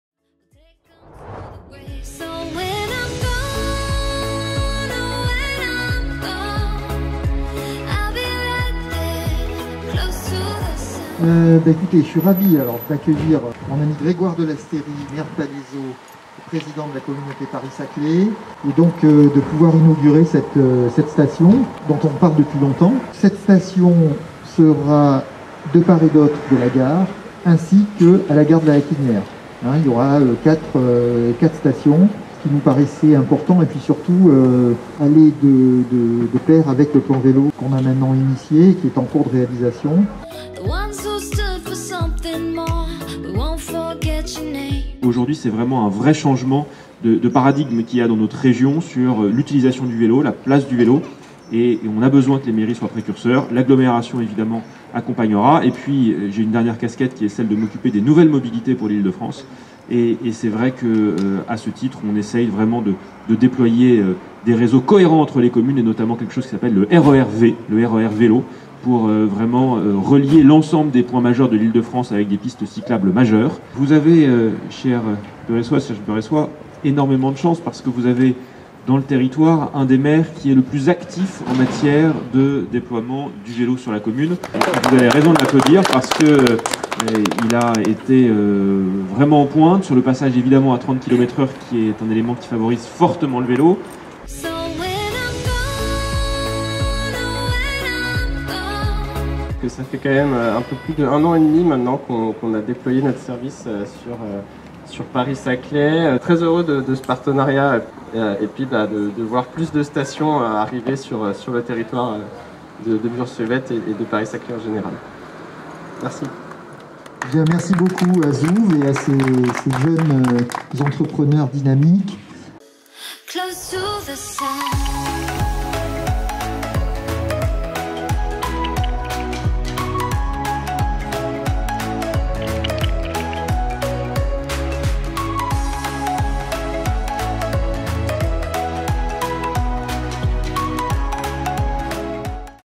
Inauguration des stations de vélos électriques ZOOV à Bures-sur-Yvette
Samedi 10 octobre 2020 les quatre stations de vélos électriques ZOOV ont été inauguré à Bures-sur-Yvette.

[ La musique de la vidéo ]

Vidéo réalisée par le service communication.
©Mairie de Bures-sur-Yvette – Octobre 2020